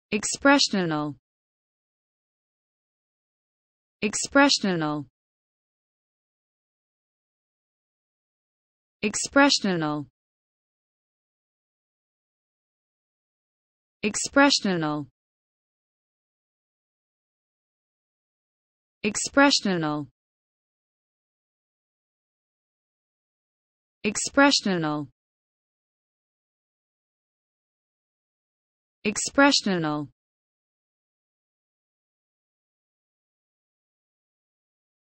the correct pronunciation of expressional in English.
This video shows you how to pronunciation expressional in English.